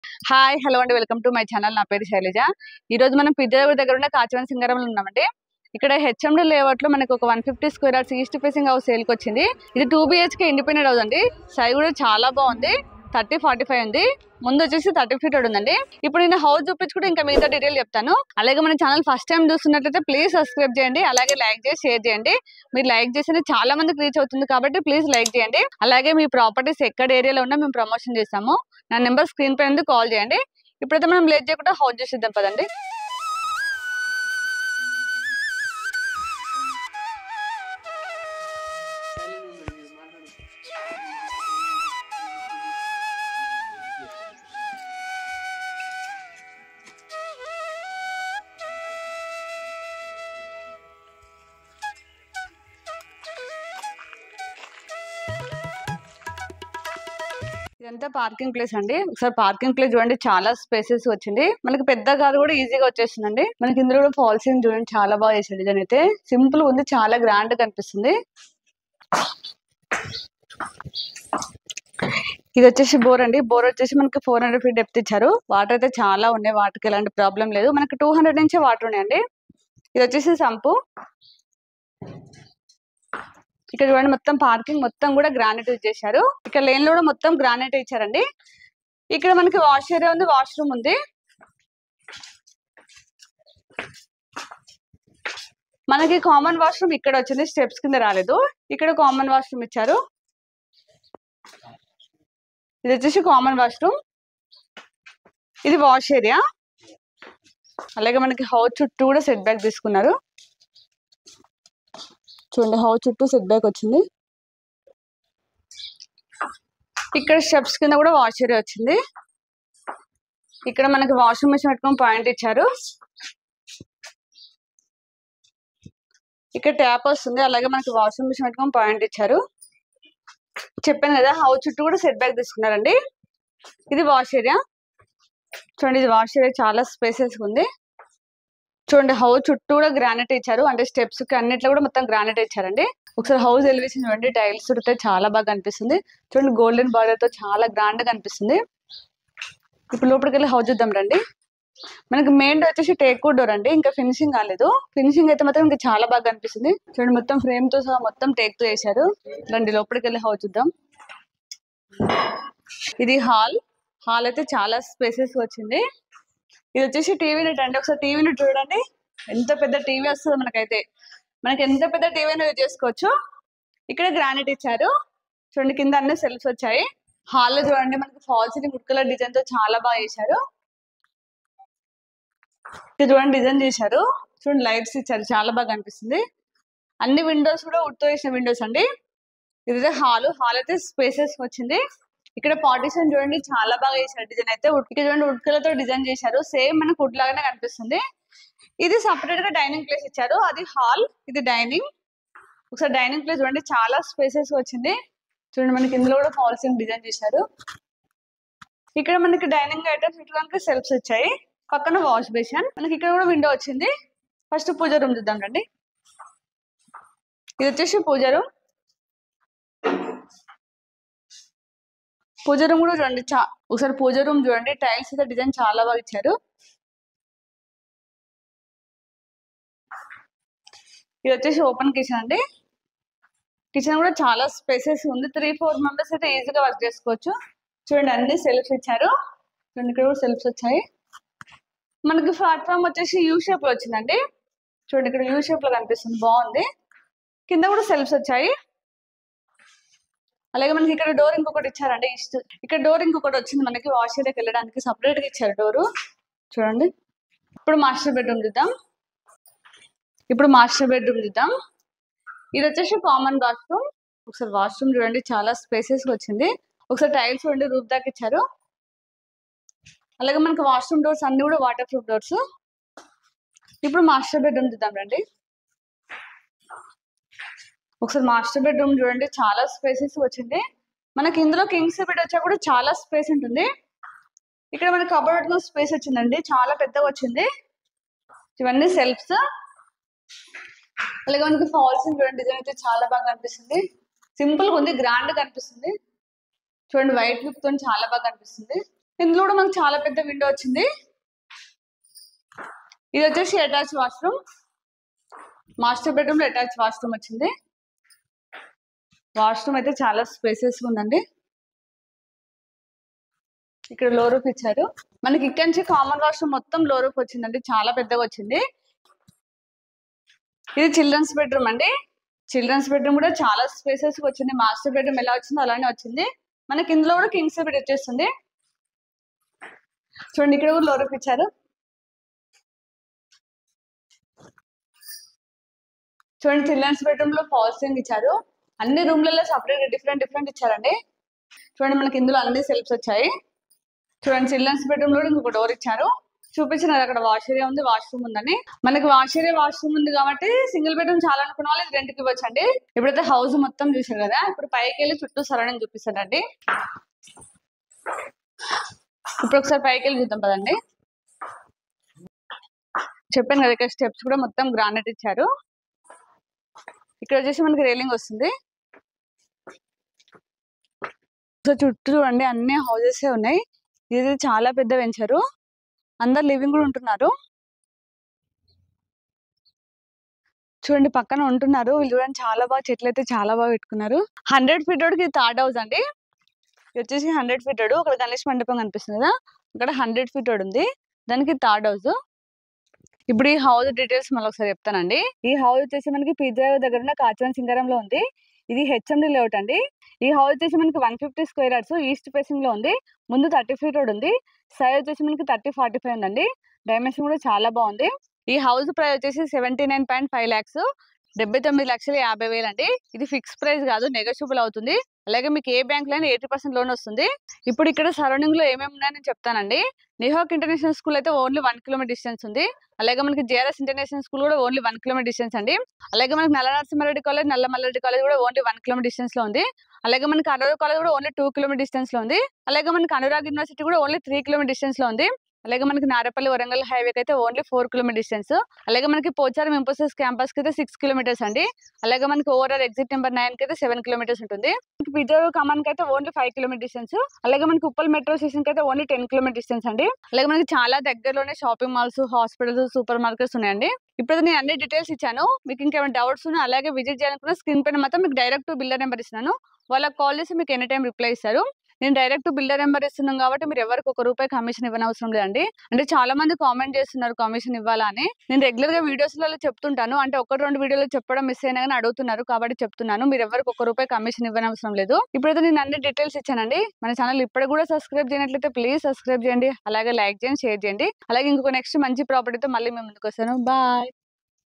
HMDA Approved 150 Square yards East Face face Low budget House Peerzadiguda ! Be Reality & Vlogs
150 గజలలో కట్టిన చక్కటి అందమైన ఇల్లు / 150 Squre yards North Face house

నమస్కారం 🙏 అండి ఇప్పుడు మీరు చుసిన ఇంటి వివరాలు పూర్తిగా తెలుసుకోవాలి అనుకుంటున్నారా.
అయితే ఈ క్రింద అన్ని వివరాలు తెలుపటం జరిగింది. దయచేసి అన్ని వివరాలు తెలుసుకొని సంప్రదించండి.

👉Any Property Promotion please Call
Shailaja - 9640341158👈

 [1] 🏠 House  Details
👉 House Code - BR142
👉Squre yards -.  150  Squre yards
👉Size -  30/45
👉Facing - East Face
👉Front Road -  30 feet
👉Water - Municipality  &  Bore
👉Permission - HMDA Approved
👉Bhk - 2bhk
👉Ground - parking
👉Loans - 80 % Approvels from any Bank

🙏Distance

** Walkable distance from Peerzadiguda 100 feet Road  Bus stop
** 2km Distance from ORR
** 4KM Distance from Warangal Highway Narapally
** 7km Distance from Infosys Pocharam Campus
** 10km Uppal Metro 🚇 Station

[2] House Location

Kachawani  Singaram Near Peerzadiguda (Uppal ) Hyderabad

Any Queries
Call Builder

Be Reality  &  Vlogs
Shailaja